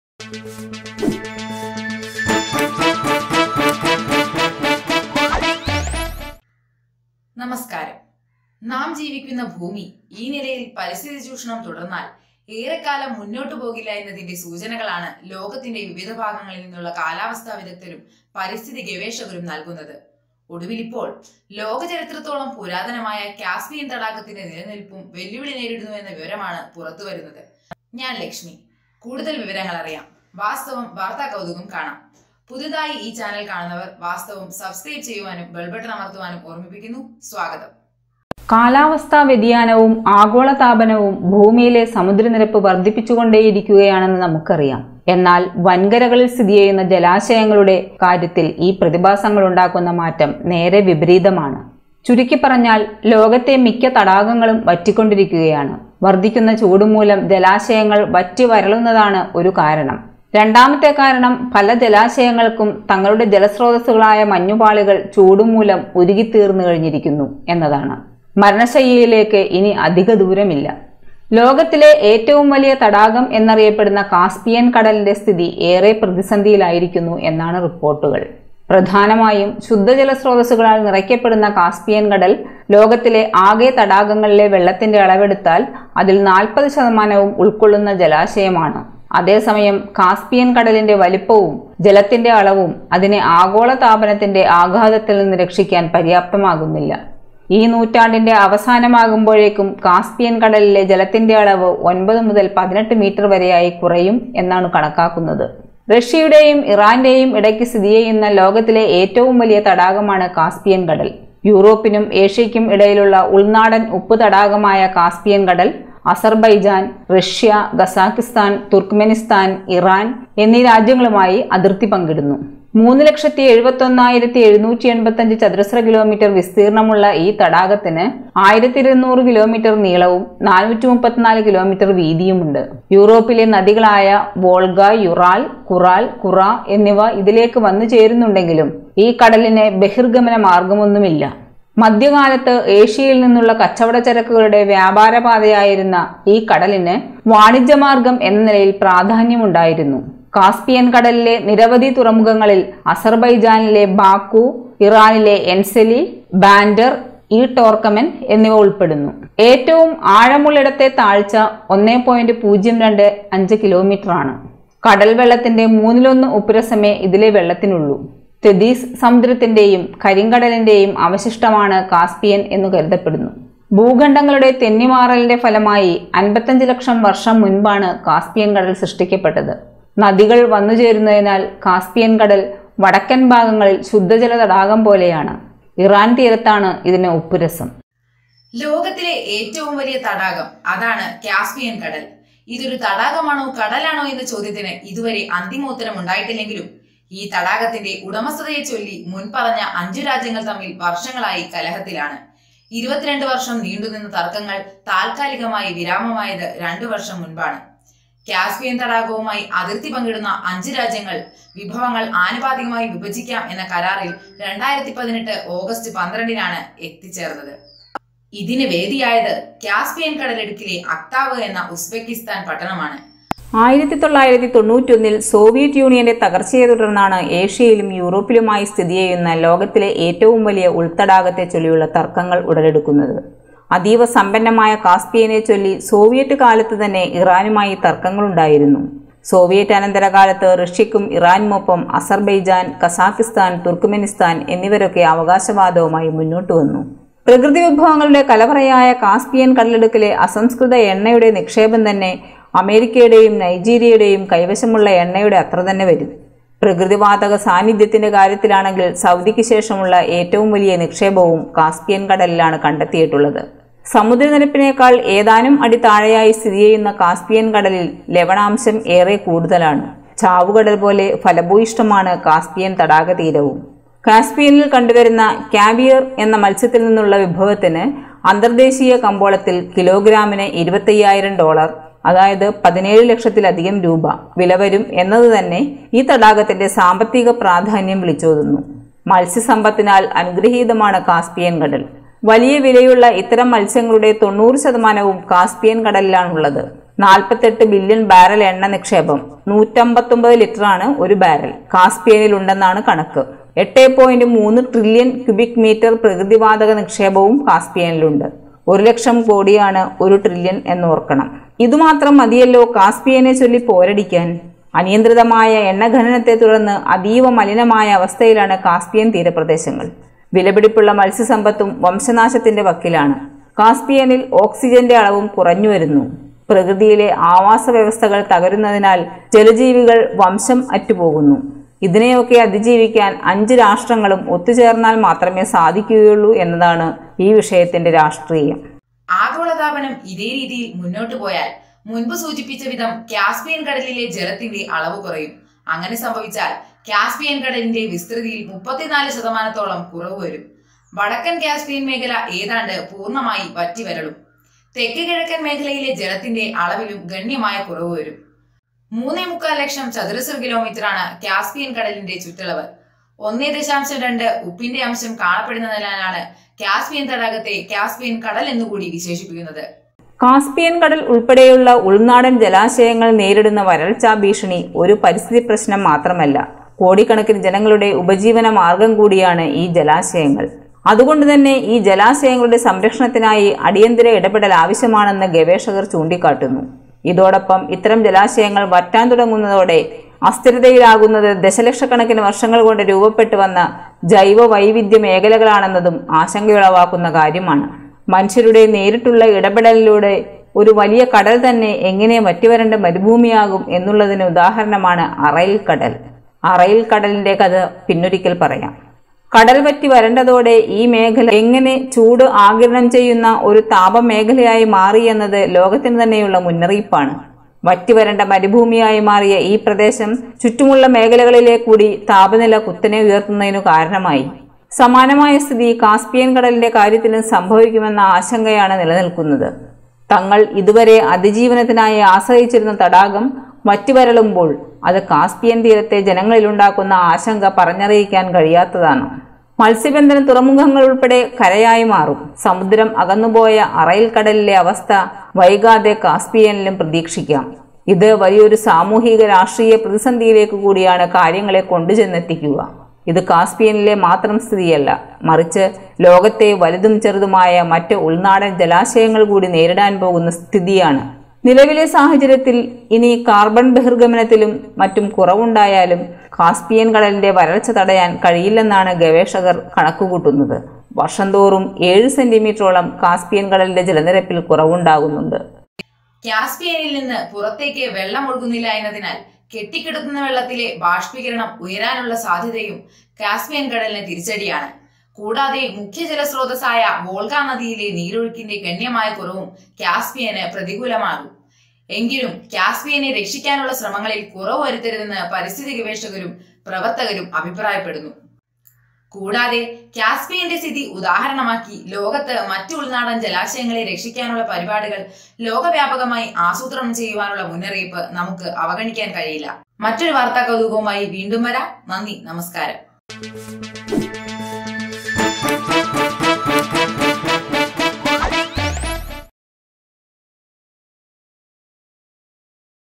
ലോകത്തെ മിക്ക തടാകങ്ങളും വറ്റിക്കൊണ്ടിരിക്കുകയാണ്. വർധിക്കുന്ന ചൂട് മൂലം /vasthavam
ലോകത്തിലെ ഏറ്റവും വലിയ തടാകം എന്നറിയപ്പെടുന്ന കാസ്പിയന്‍ കടലിന്‍റെ സ്ഥിതി ഏറെ പ്രതിസന്ധിയിൽ ആയിരിക്കുന്നു എന്നാണ് റിപ്പോർട്ടുകൾ. പ്രധാനമായും ശുദ്ധജലസ്രോതസ്സുകളാല്‍ നിറയ്ക്കപ്പെടുന്ന കാസ്പിയന്‍ കടല്‍ ലോകത്തിലെ ആകെ തടാകങ്ങളിലെ വെള്ളത്തിന്‍റെ അളവെടുത്താല്‍, അതില്‍ 40 ശതമാനവും ഉള്‍ക്കൊള്ളുന്ന ജലാശയമാണ്. അതേസമയം കാസ്പിയന്‍ കടലിന്‍റെ വലുപ്പവും ജലത്തിന്‍റെ അളവും അതിനെ ആഗോളതാപനത്തിന്‍റെ ആഘാതത്തില്‍ നിന്ന് രക്ഷിക്കാന്‍ പര്യാപ്തമാകുന്നില്ല. ഈ നൂറ്റാണ്ടിന്‍റെ അവസാനമാകുമ്പോഴേക്കും കാസ്പിയന്‍ കടലിലെ ജലത്തിന്‍റെ അളവ്  9 മുതല്‍ 18 മീറ്റര്‍ വരെയായി കുറയും എന്നാണു കണക്കാക്കുന്നത്.